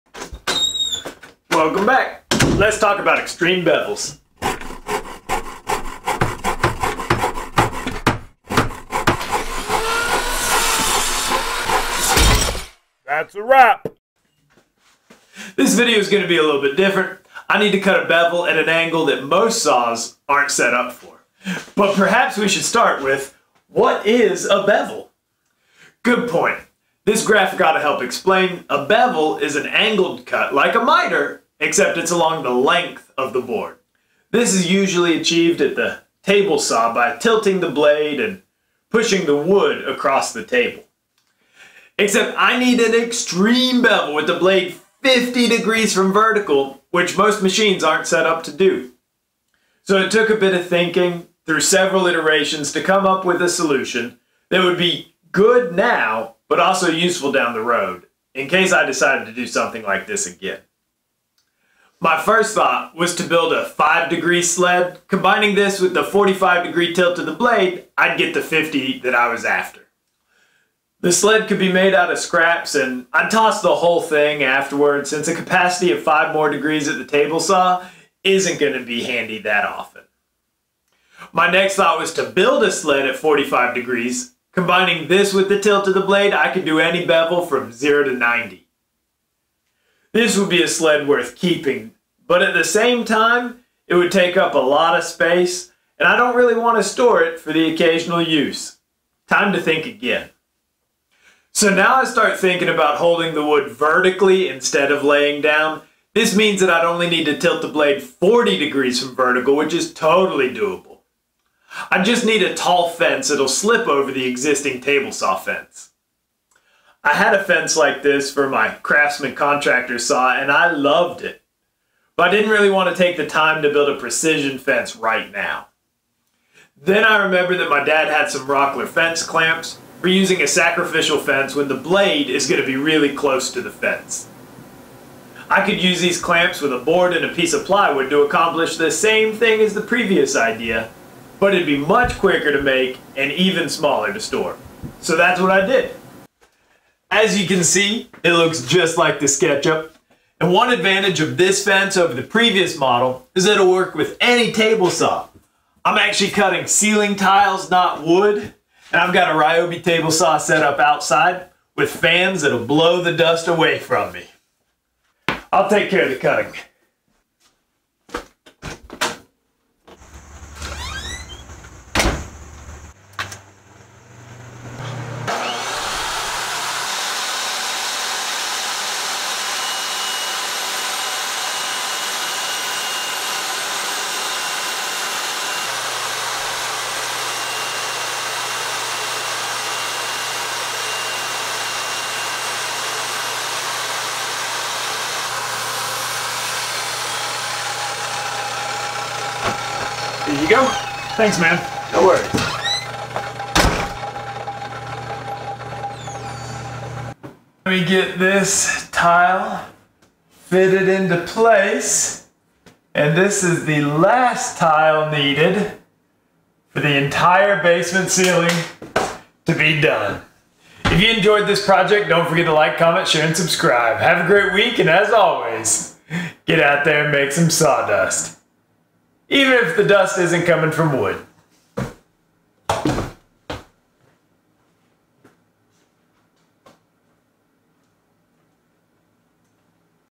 Extreme Bevels
First, I have to apologize for the shots that are out of focus. It has been a month since my last upload and I just wanted to get this video done. I hope you enjoy hearing my thought process as I figured out the easiest way to make this extreme bevel cut.